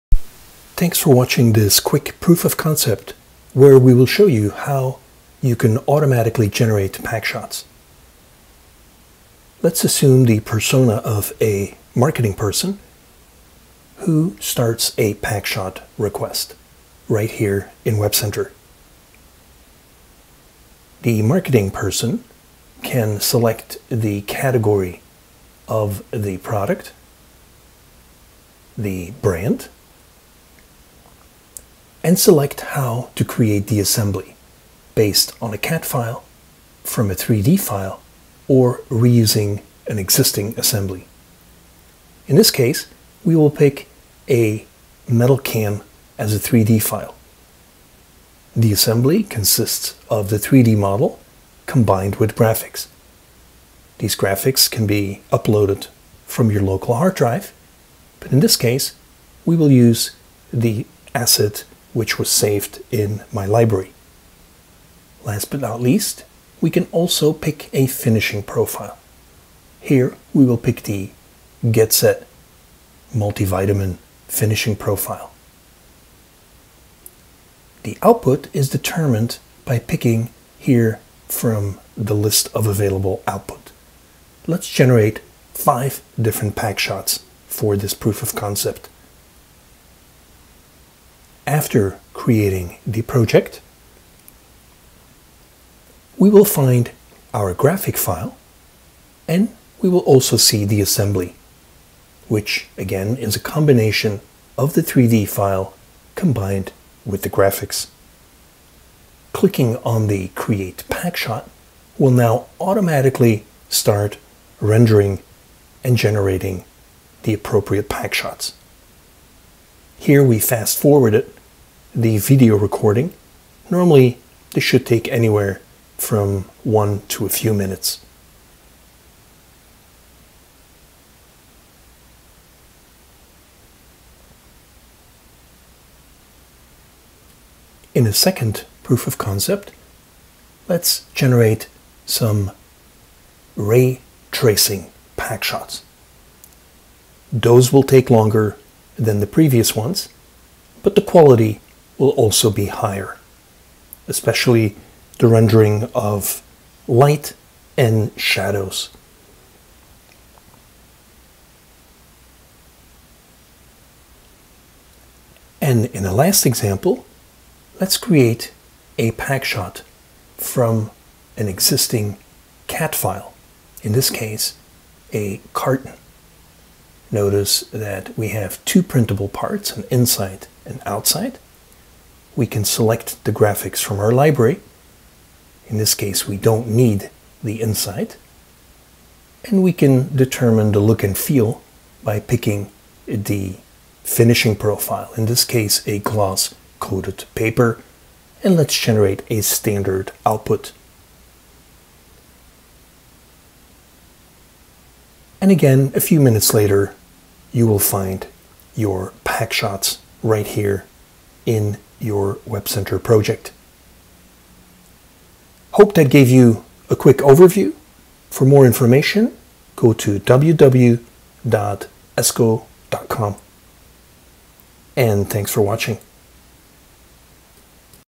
Esko Automated Packshots with WebCenter.
Quick proof of concept to generate Packshots